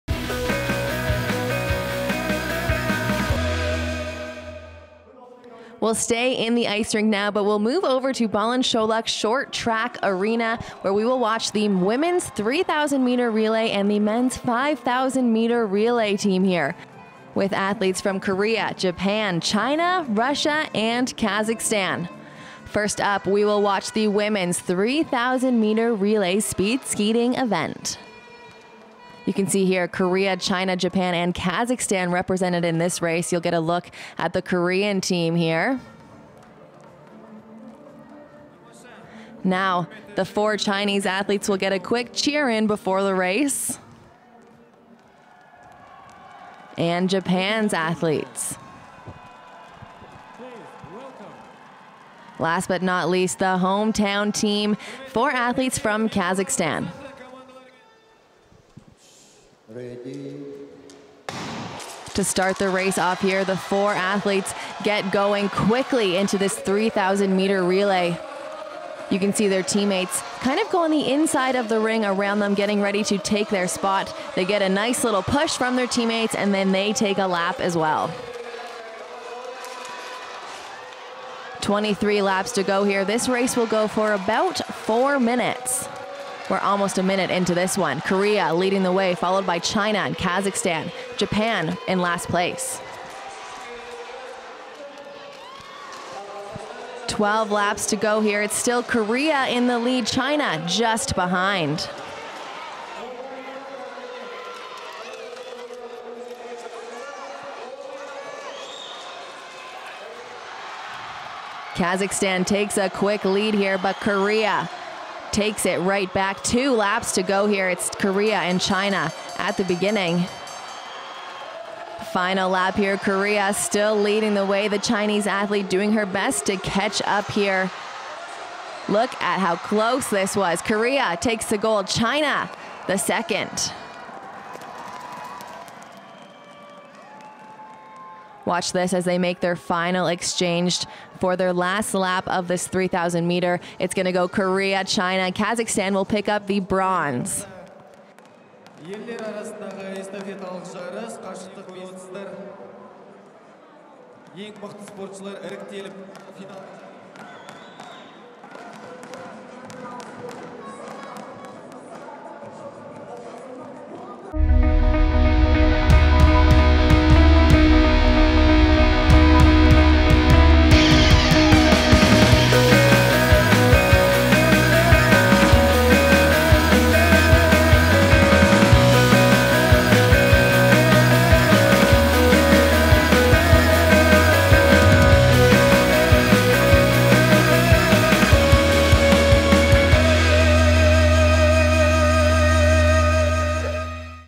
Short track speed skating 3000m Women's relay - 28th Winter Universiade 2017, Almaty, Kazakhstan
Almaty: Korean short track speed skating team won the relay race

February 7. Women’s short track speed skating. 3000m relay. Final A:

1. Korea - 4:13.630

2. China - 4:13.808

3. Kazakhstan - 4:18.369

Top three teams of 3000m relay race were determined in short track speed skating. Four teams participated in the decisive race. Korea and China competed for gold. Both teams led the race by far compared to other athletes. In the end, Korean athletes won, while China came second. Athletes from Kazakhstan were third, having outran their Japanese rivals.